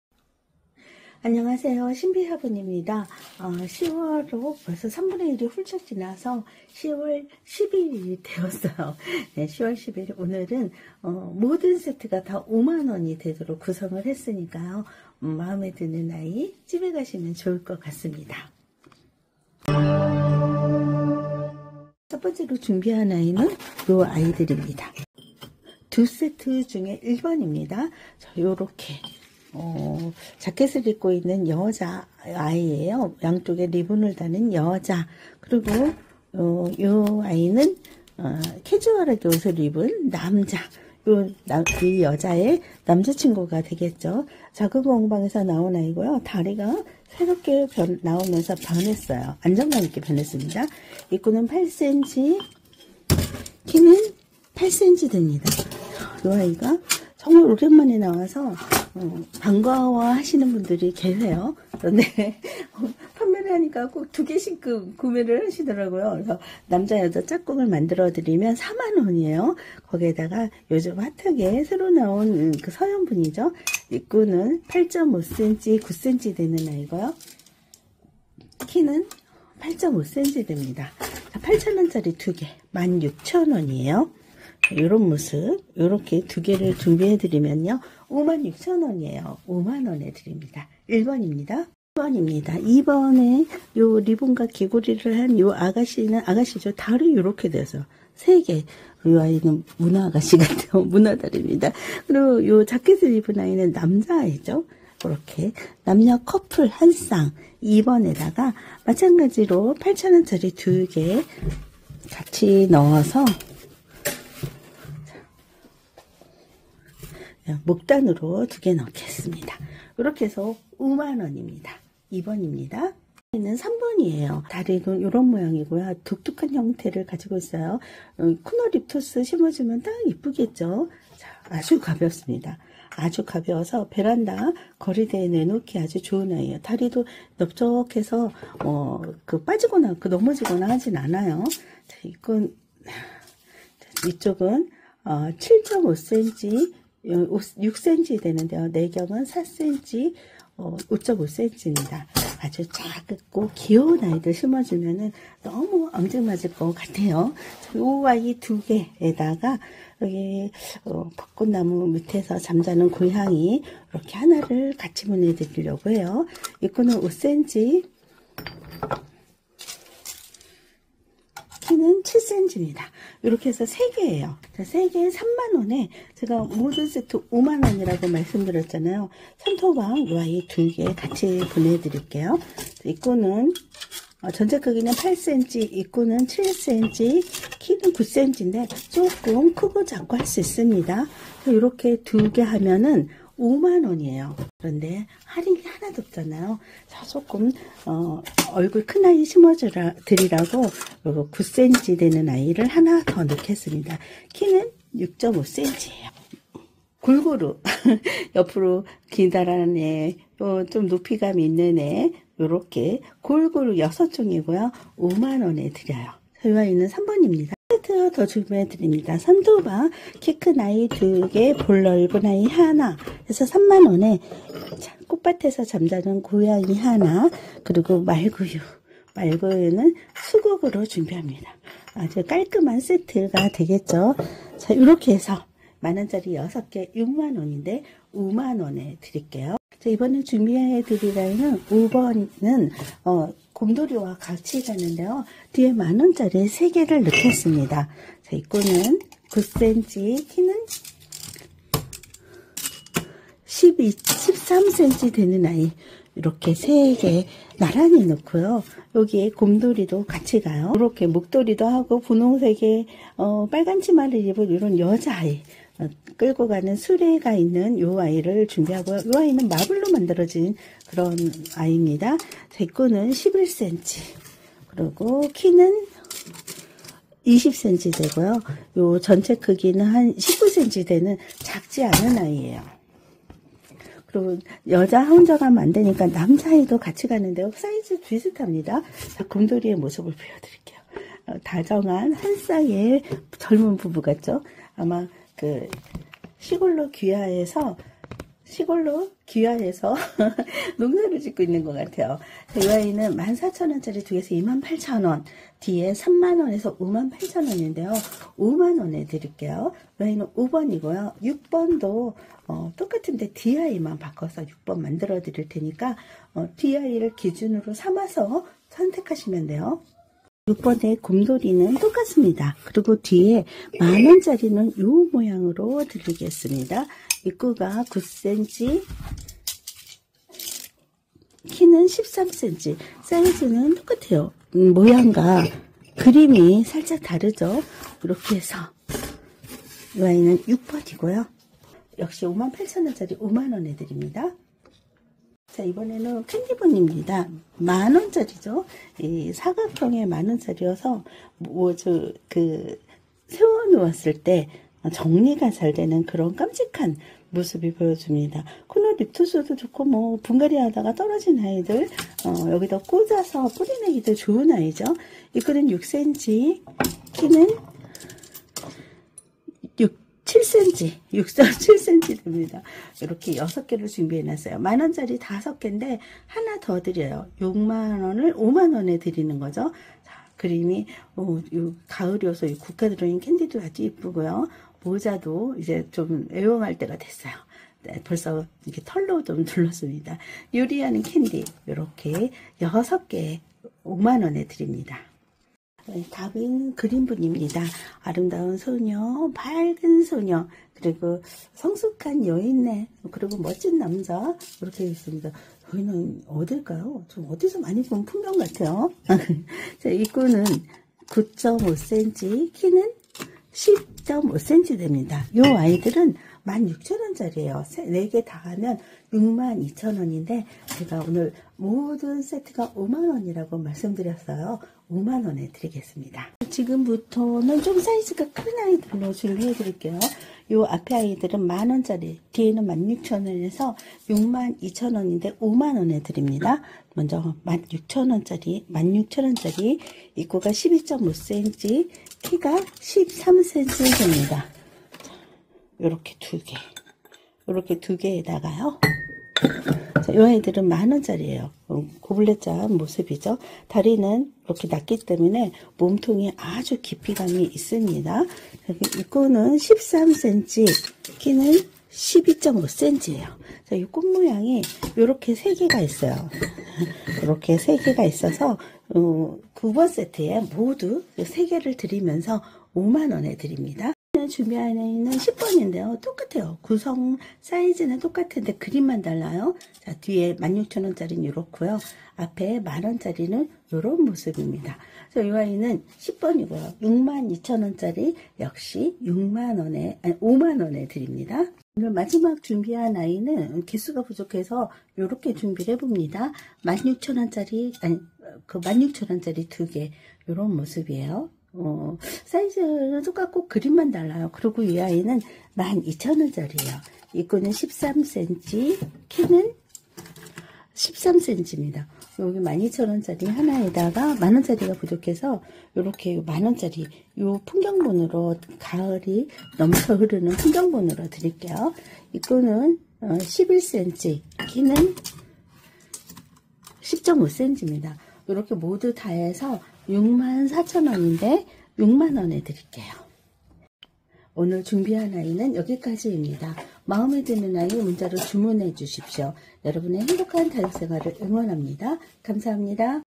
신비화분(신비의다육정원)- 묻지도 따지지도 말고 무조건 몽땅 5만원
저렴한아이 착한가격 자기야 청기와 화분과 예술성이 듬뿍인 명품화분 골라가세요
저렴한 가격으로 자기야 청기와 등을 준비했습니다.
★단순변심반품은 반품되지 않습니다
★도서산간지역은 택배비가 추가될수 있습니다.